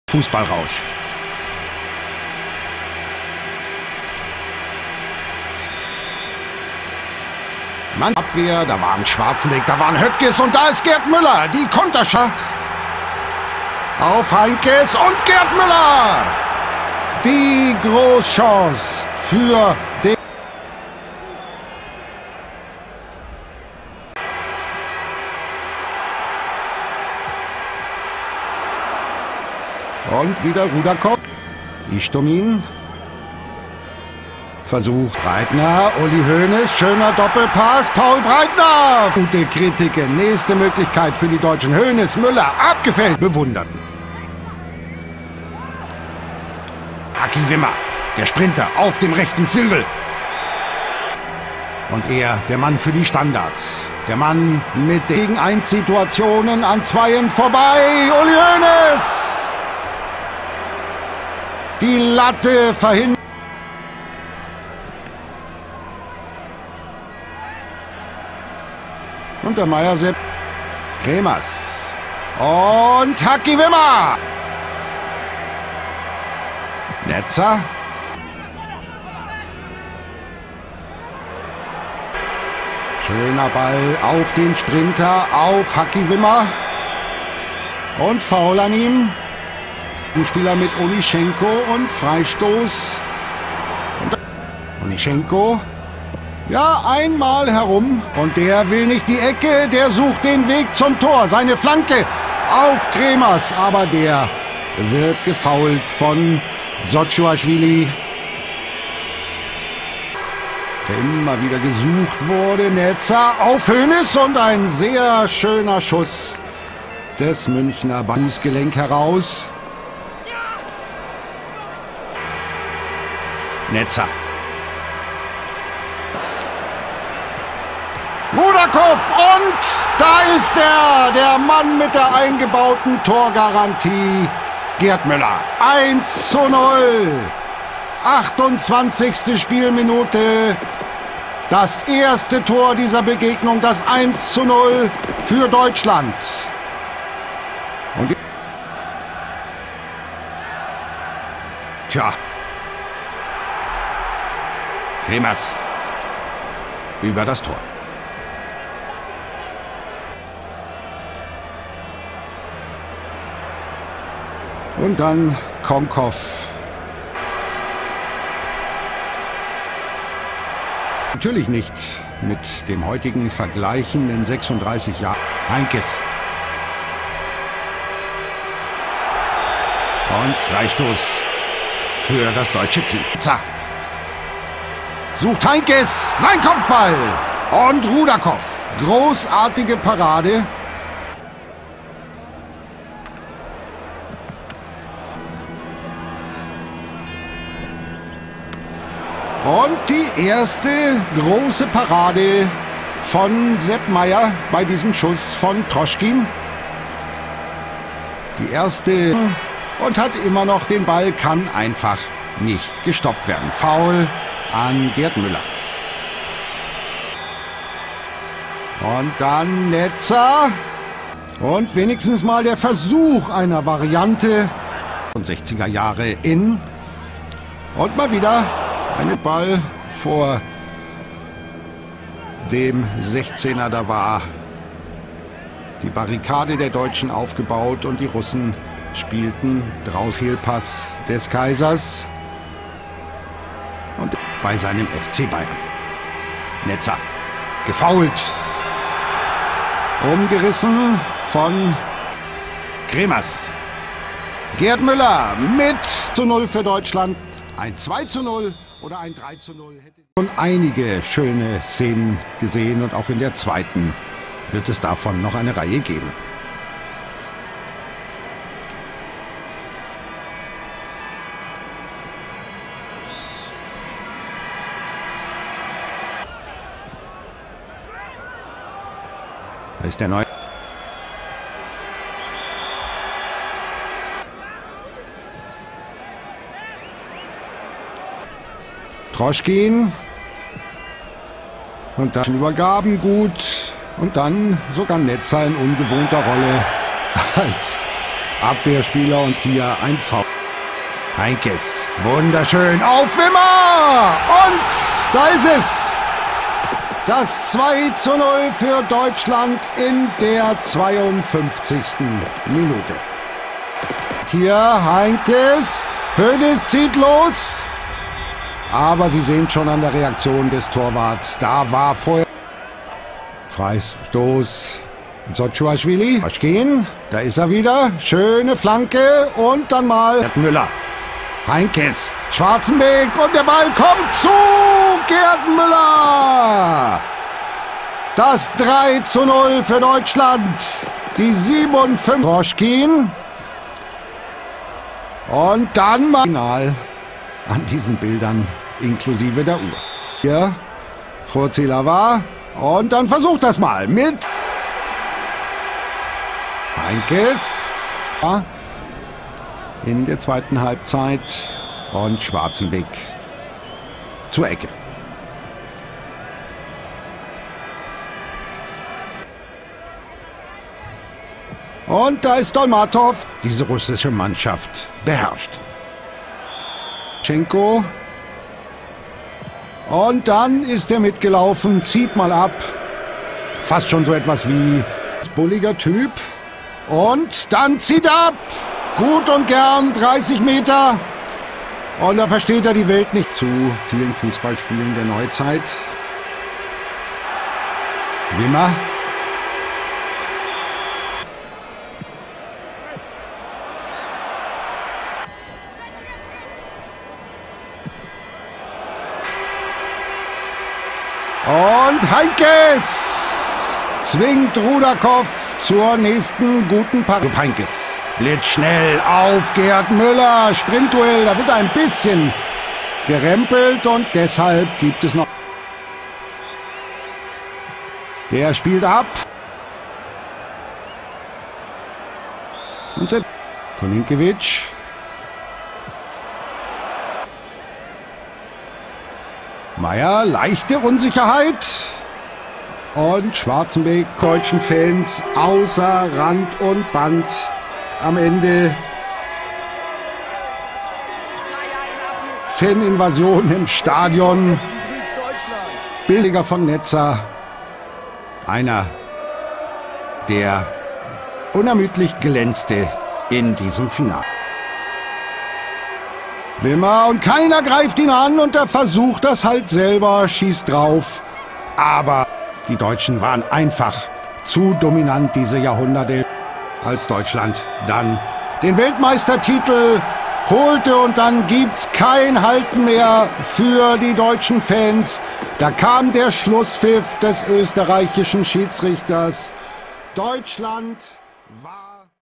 West Germany vs Soviet Union 3-0 All Goals & Highlights ( 1972 UEFA EURO Final )
18 June 1972 | 16:00

            West Germany 3–0 Soviet Union

Müller  27', 58'

Wimmer  52'

Heysel Stadium, Brussels

Attendance: 43,437